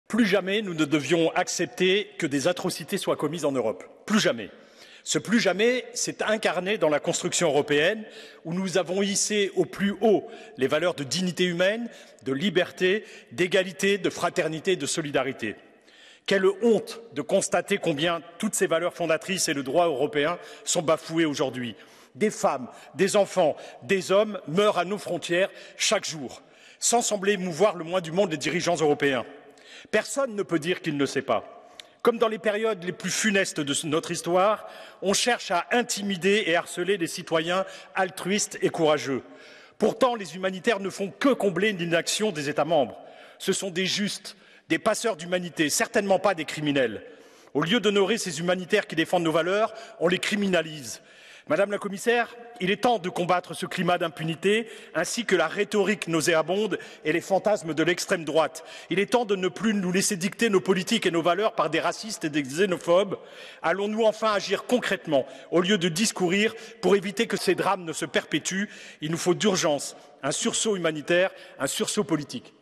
Damien Carême sur la criminalisation de l'aide humanitaire
Mercredi 18 janvier, Damien Carême est intervenu sur la criminalisation de l'aide humanitaire.

Son intervention ⬇

"Plus jamais nous ne devions accepter que des atrocités soient commises en Europe. Plus jamais. Ce "plus jamais" s'est incarné dans la construction européenne où nous avons hissé au plus haut les valeurs de dignité humaine, de liberté, d'égalité, de fraternité, de solidarité. Quelle honte de constater combien toutes ces valeurs fondatrices et le droit européen sont bafoués aujourd'hui.
Des femmes, des enfants, des hommes meurent à nos frontières, chaque jour, sans sembler émouvoir le moins du monde les dirigeants européens. Personne ne peut dire qu'il ne sait pas. Comme dans les périodes les plus funestes de notre histoire, on cherche à intimider et harceler des citoyens altruistes et courageux. Pourtant, les humanitaires ne font que combler l'inaction des États membres. Ce sont des justes, des passeurs d'humanité. Certainement pas des criminels. Au lieu d'honorer ces humanitaires qui défendent nos valeurs, on les criminalise.
Mme la Commissaire, il est temps de combattre ce climat d'impunité ainsi que la rhétorique nauséabonde et les fantasmes de l'extrême droite. Il est temps de ne plus nous laisser dicter nos politiques et nos valeurs par des racistes et des xénophobes.
Allons nous enfin agir concrètement au lieu de discourir ? Pour éviter que ces drames ne se perpétuent, il nous faut d'urgence un sursaut humanitaire, un sursaut politique."